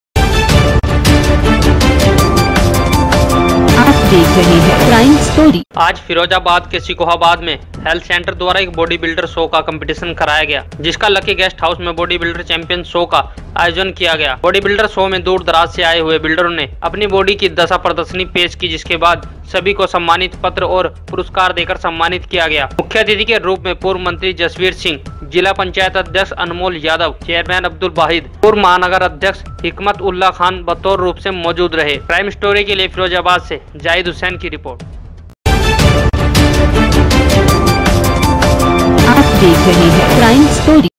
बॉडी बिल्डिंग चैंपियनशिप का आयोजन
( क्राइम स्टोरी के लिए उत्तर प्रदेश के फिरोजाबाद से जाहिद हुसैन की रिपोर्ट I )